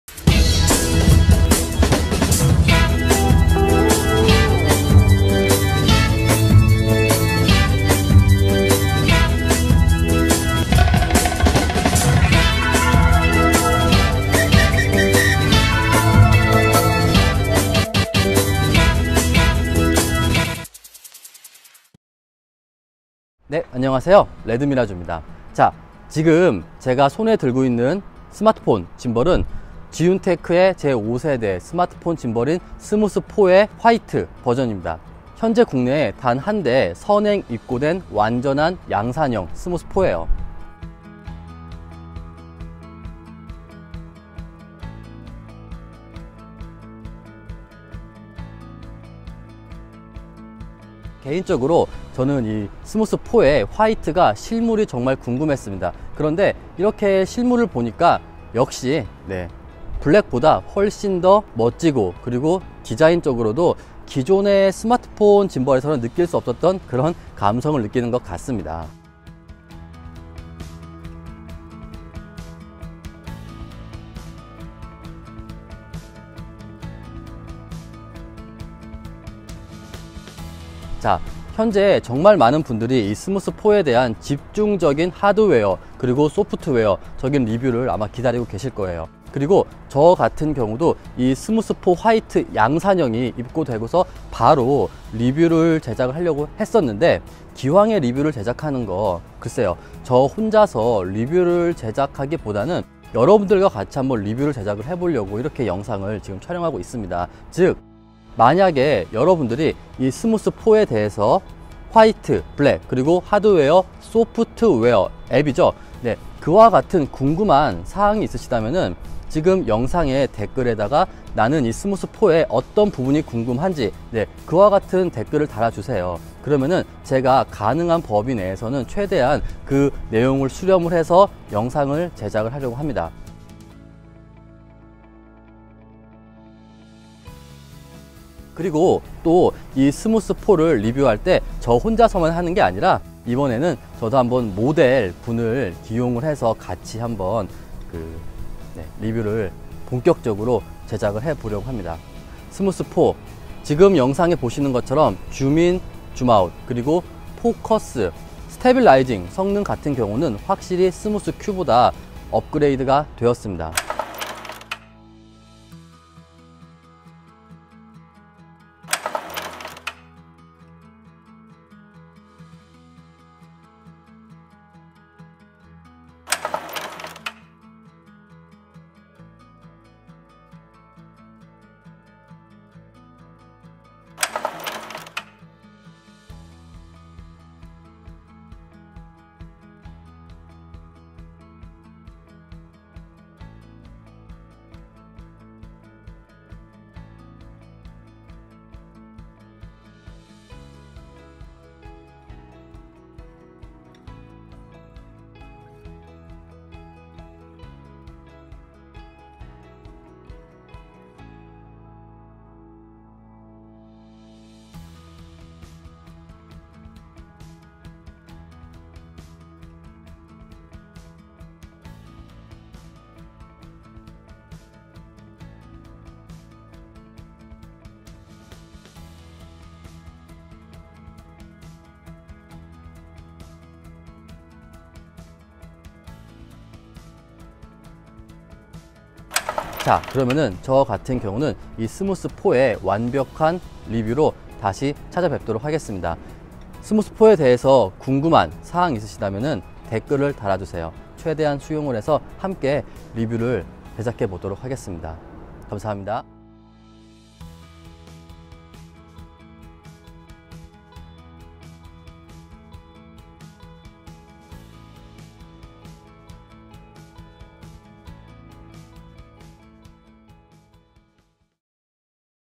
5세대 스마트폰 짐벌 스무스4의 리뷰를 함께 제작하고 싶습니다/Smooth4
안녕하세요.
레드미라쥬입니다.

현재 정말 많은 분들이 스무스4의 본격적인 리뷰 영상을 기다리고 계실겁니다.

본 영상은 지윤테크의 제5세대 스마트폰 짐벌인 스무스4 화이트의 최초 공개와 간단한 프리뷰 영상입니다. 완전한 양산형 스무스4가 입고 되고 바로 본격 리뷰를 제작하려고 했습니다.

그런데 본격적으로 리뷰는 제작하는 부분에서 여러분들이 의견을 최대한 수렴한 스무스4의 리뷰를 제작하면 어떨까 하는 생각이 들었습니다. 그리고 저 혼자 등장하는 것이 아닌 아름다운 모델분과 더욱 멋진 리뷰를 제작 하기위해 다음주에 스무스4의 본격 리뷰를 촬영 하기로 했습니다.

(물론 이는 스무스4용 ZY PLAY앱과 관련도 있습니다. 신형 ZY PLAY앱은 4월 중순 업데이트가 될 입니다.)

그리고 많은 분들께서 스무스4의 본격 리뷰를 기다리고 계시기에 본 영상에서는 스무스4의 실제 운용 영상과 스무스Q, 스무스3의 비교 영상을 담아봤습니다.

만약 스무스4에 관하여 궁금한 사항이 있다면 댓글로 남겨 주십시오.
최대한 여러분들의 의견을 수렴한 본격 통합 리뷰를 제작해 보도록 하겠습니다.

감사합니다